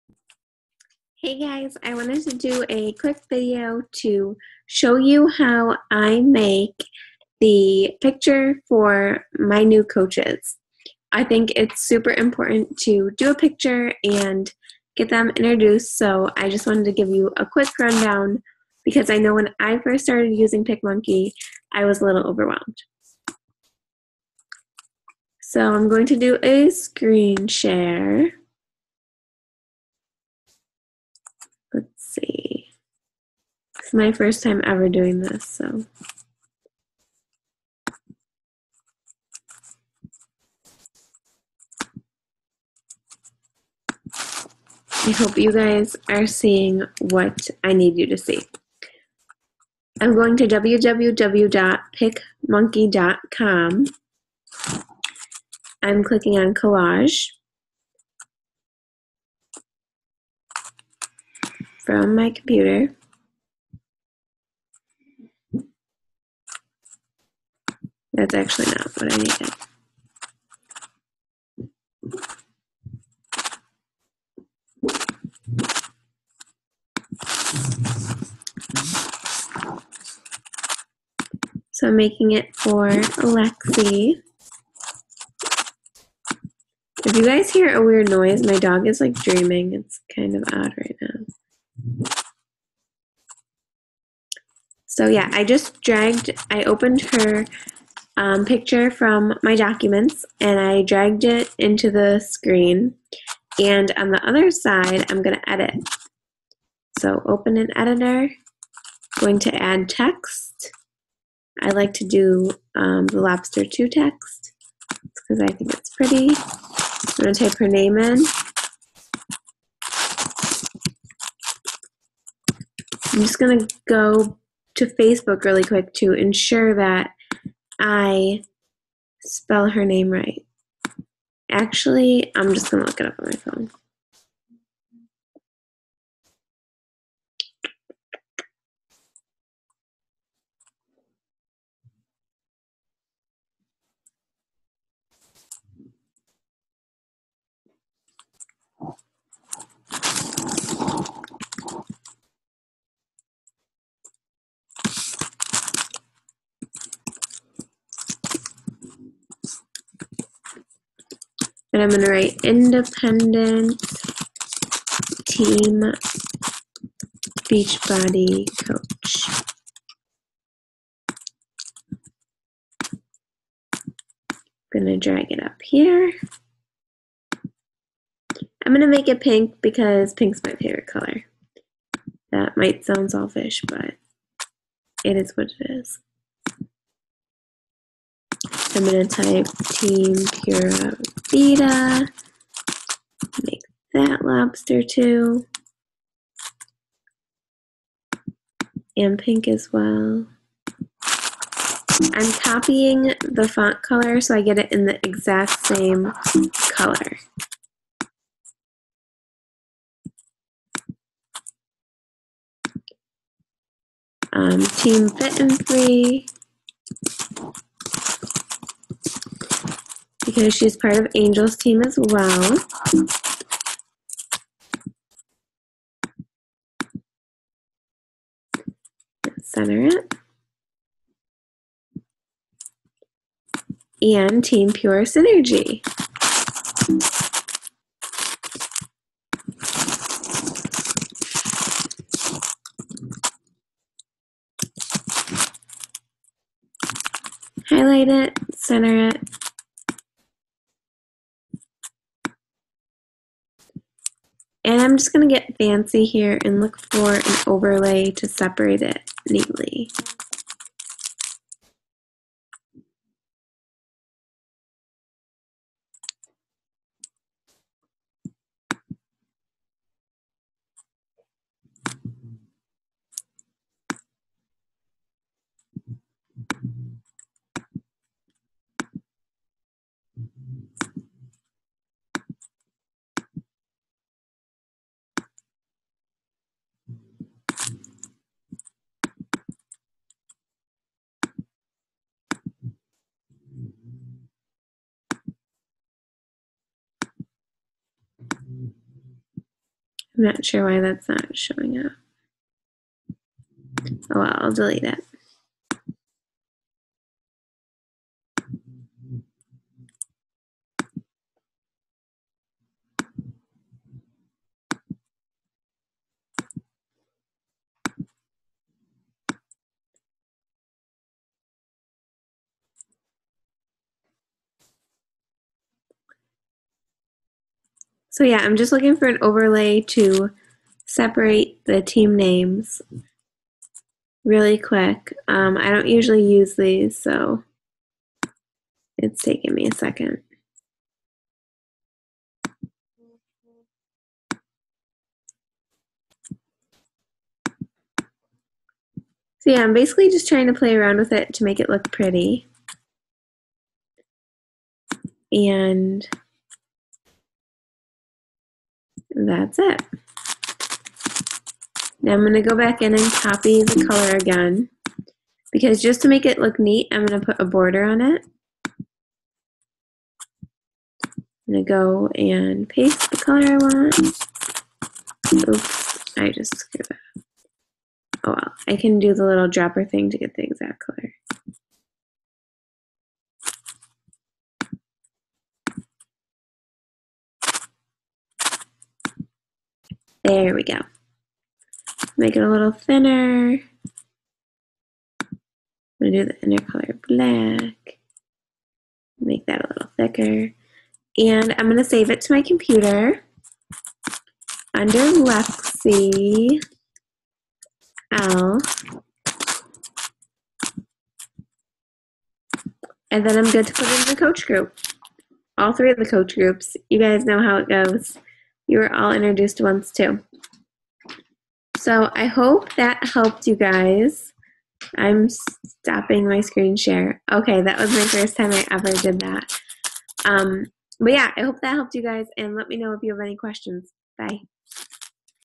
How I make my new coach intro photos
I use to make my new coach introduction photos! Here's what I do!